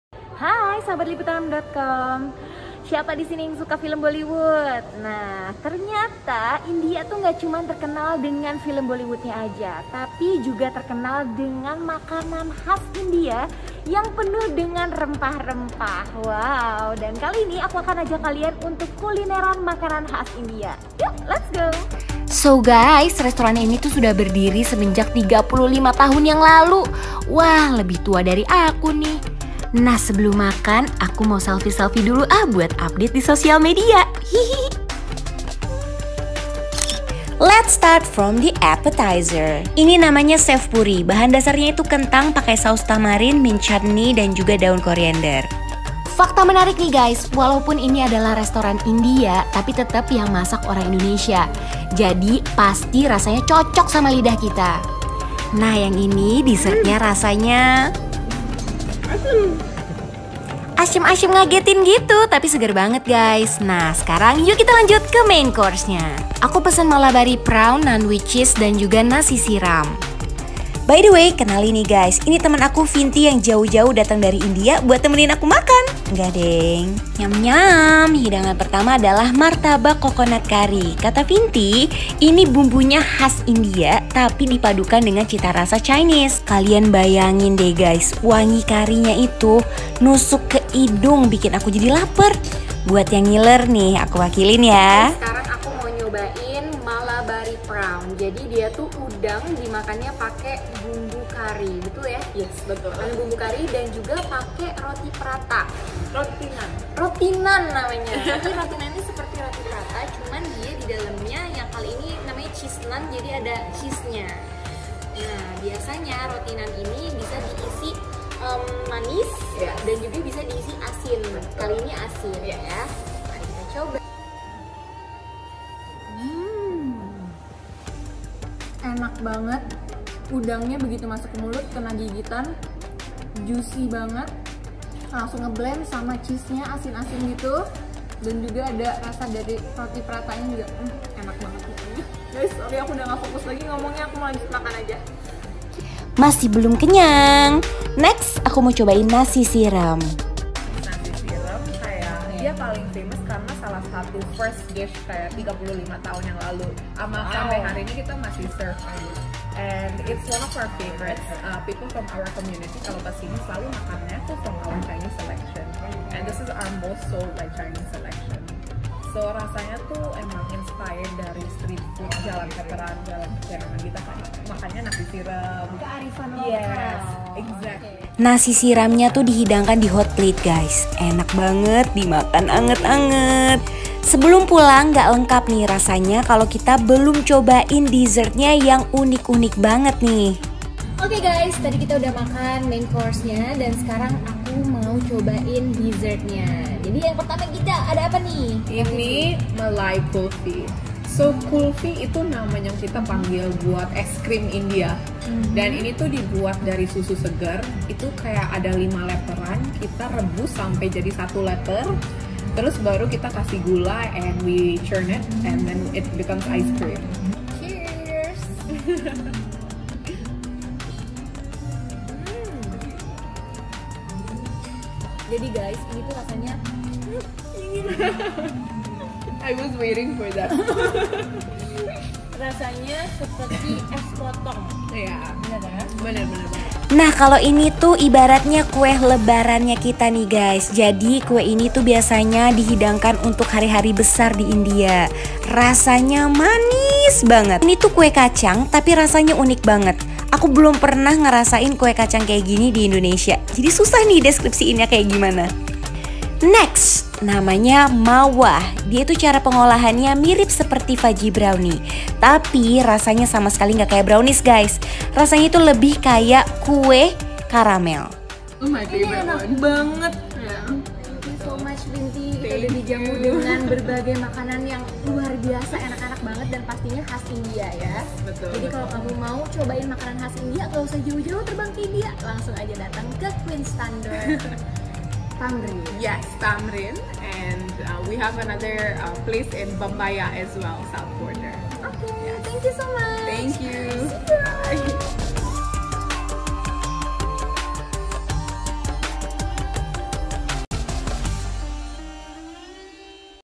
Merasakan Kuliner Mewah Khas India di Jakarta - Liputan6.com
Siapa di antara kamu yang suka banget sama kebudayaan India? Nah kali ini, kita bakal ajak kamu untuk mencicipi kuliner mewah khas India yang bisa ditemui di Jakarta.